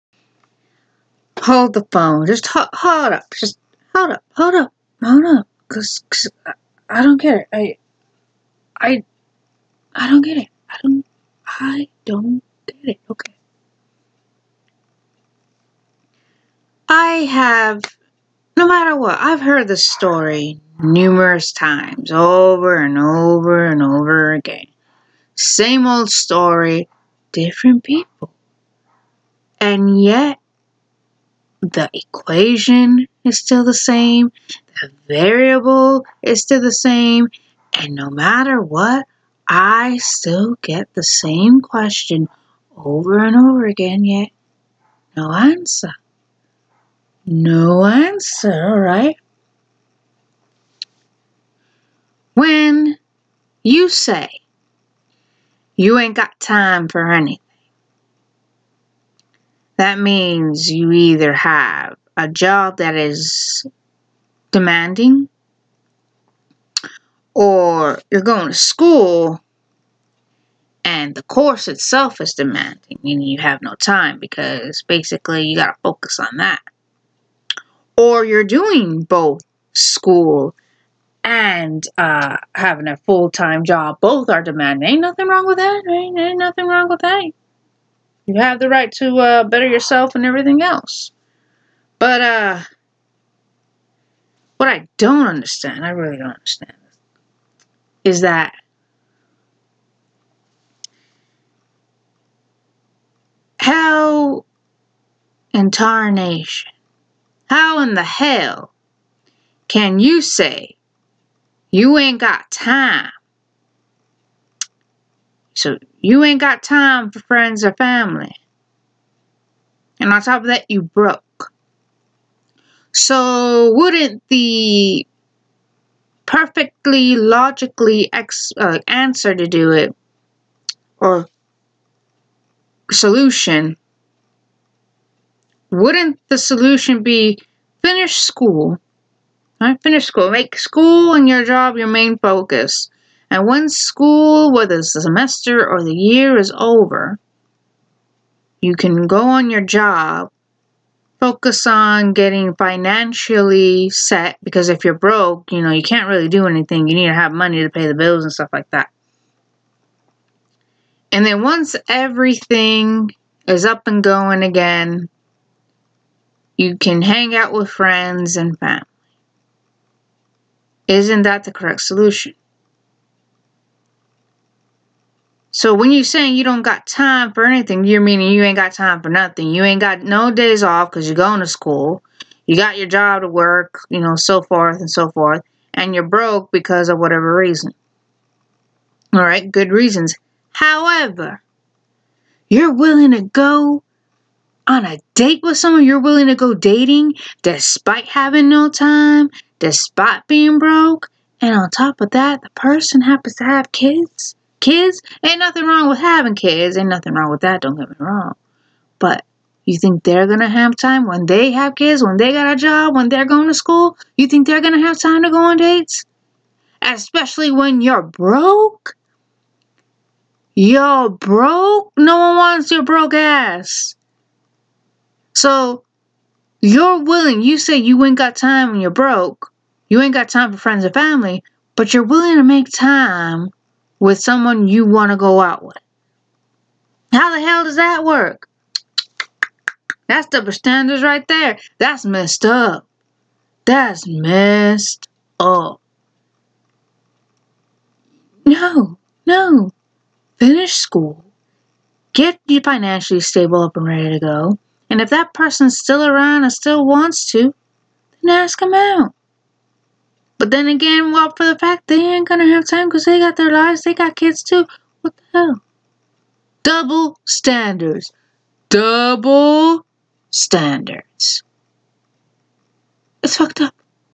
Get your life!
Don't be saying you ain't got time for nothing so you can't hang out with friends or family and yet you got time to date around. You a hypocrite get your life!